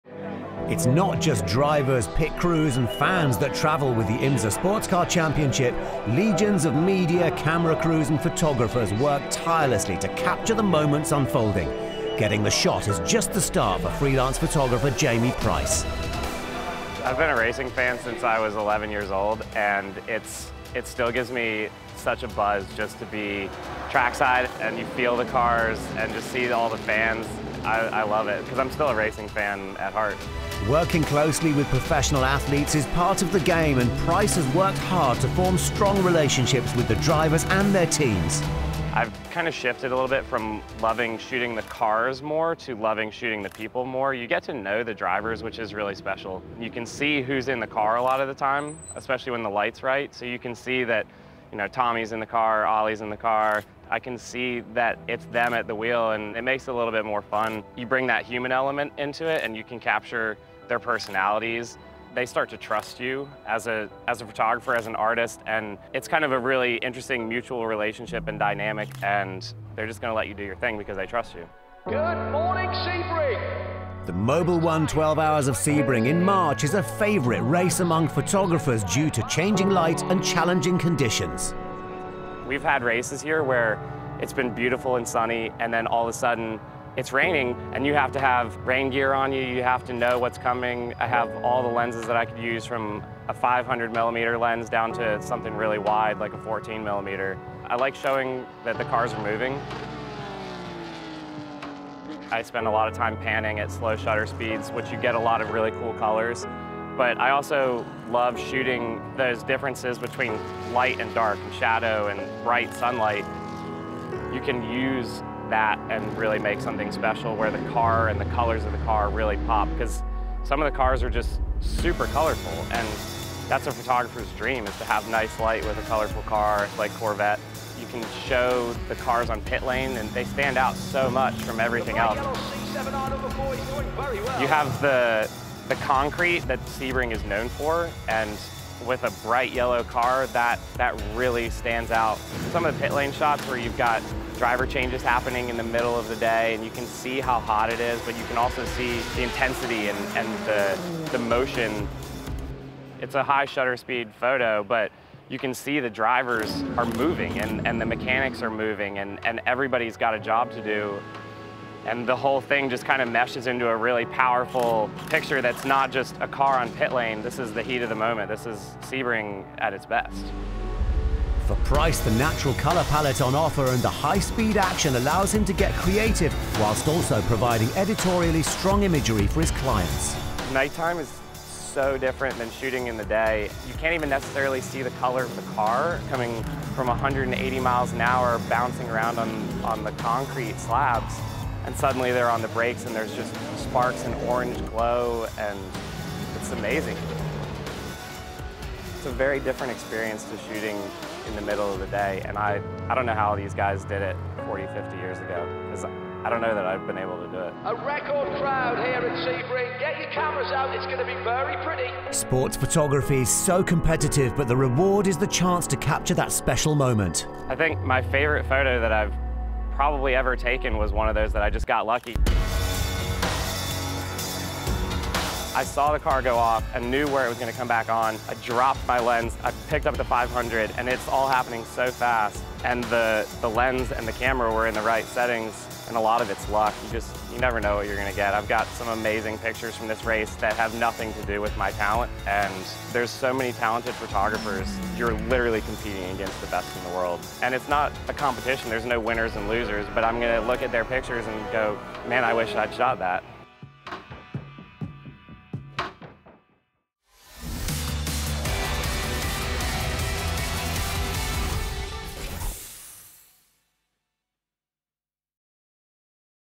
Snapshot – Motorsport Photographer Jamey Price On Capturing The Moment | M1TG
Jamey Price has marked himself out as one of the leading photographers on the sportscar circuit, and across the wider motor racing scene, with his talent and vision for the perfect shot. Mobil 1 The Grid spent time with the American at the recent Mobil 1 12 Hours of Sebring to learn more about his craft and how he approaches such a unique day-to-night event.